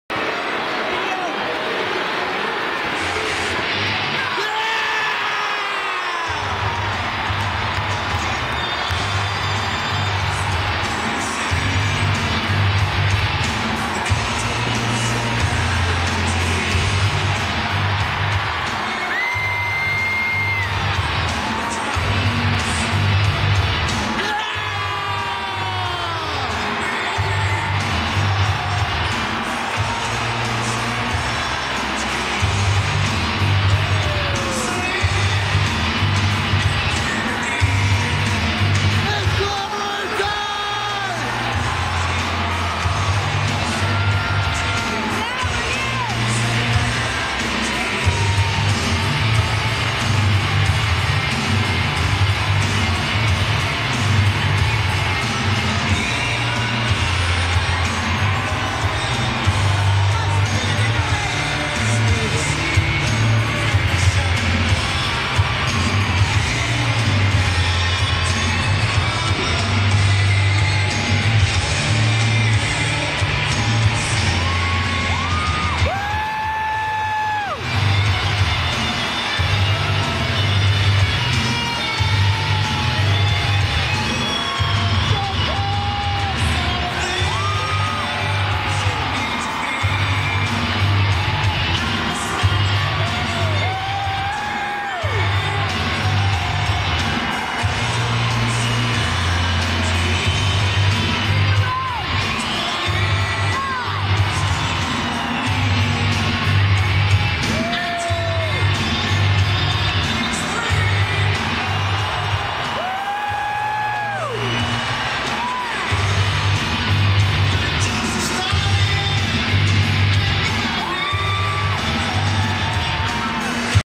CM Punk's Entrance Live Toronto March 9th
Cm punk's entrance at the WWE raw wrestlemania tour March 9th 2012